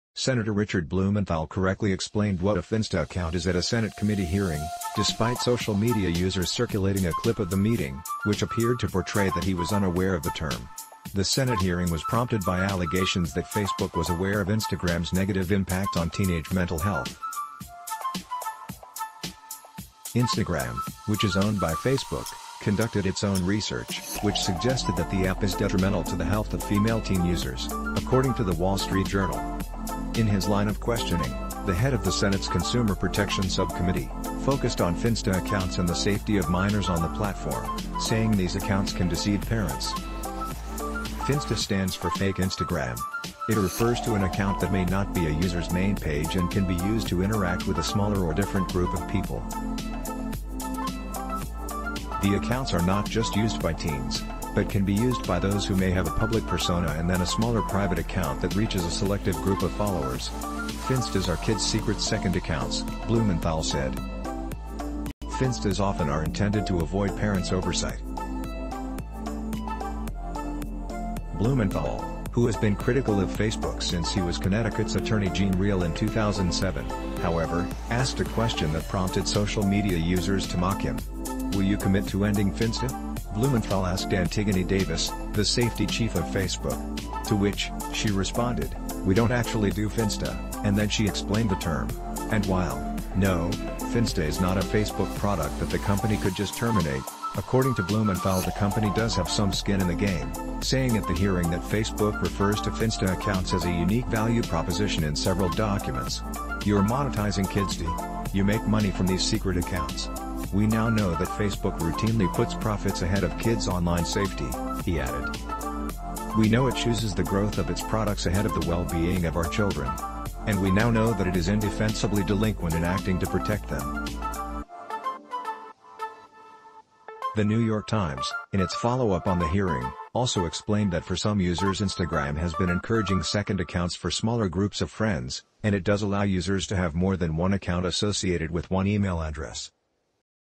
Sen Blumenthal explains 'finsta' and claims 'Facebook routinely puts
Sen. Richard Blumenthal correctly explained what a "finsta" account is at a Senate committee hearing, despite social media users circulating a clip of the meeting, which appeared to portray that he was unaware of the term. The Senate hearing was prompted by allegations that Facebook was aware of Instagram's negative impact on teenage mental health. Instagram, which is owned by Facebook, conducted its own research, which suggested that the app is detrimental to the health of female teen users, according to The Wall Street Journal. In his line of questioning, the head of the Senate's Consumer Protection Subcommittee, focused on "finsta" accounts and the safety of minors on the platform, saying these accounts can deceive parents. "Finsta" stands for "fake Instagram. " It refers to an account that may not be a user's main page and can be used to interact with a smaller or different group of people. The accounts are not just used by teens, but can be used by those who may have a public persona and then a smaller private account that reaches a selective group of followers."'Finstas' are kids' secret second accounts, " Blumenthal said. "Finstas often are intended to avoid parents' oversight. "Blumenthal, who has been critical of Facebook since he was Connecticut's attorney gene real in 2007, however, asked a question that prompted social media users to mock him."Will you commit to ending 'finsta? '" Blumenthal asked Antigone Davis, the safety chief of Facebook. To which, she responded, "We don't actually do 'finsta, '" and then she explained the term. And while, no, "finsta" is not a Facebook product that the company could just terminate, according to Blumenthal the company does have some skin in the game, saying at the hearing that Facebook refers to "finsta" accounts as "a unique value proposition" in several documents."You're monetizing kids' deceiving their parents, " he said. "You make money from these secret accounts. ""We now know that Facebook routinely puts profits ahead of kids' online safety, " he added. "We know it chooses the growth of its products ahead of the wellbeing of our children. And we now know that it is indefensibly delinquent in acting to protect them. "The New York Times, in its follow-up on the hearing, also explained that for some users Instagram has been encouraging second accounts for smaller groups of friends; and it does allow users to have more than one account associated with one email address. Insider reached out to Facebook regarding Blumenthal's line of questioning and the prompts that some users have recieved to open second accounts.